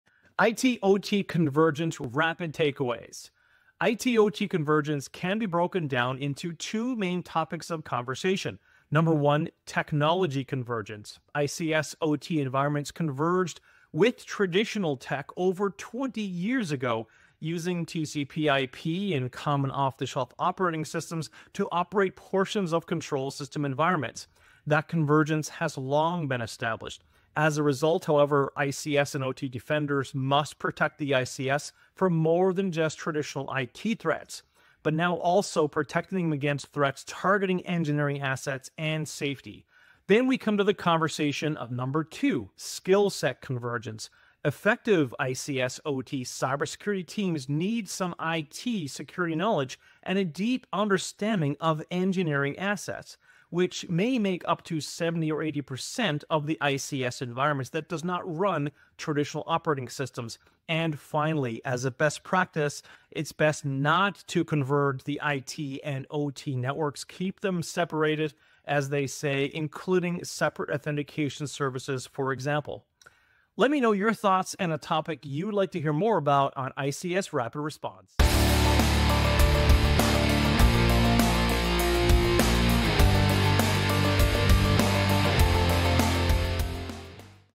IT/OT Convergence | ICS Rapid Response #9
IT/OT convergence can be broken down into two main topics of conversation: technology convergence and skillset convergence. Dean Parsons, SANS Principal Instructor breaks it down in this short video.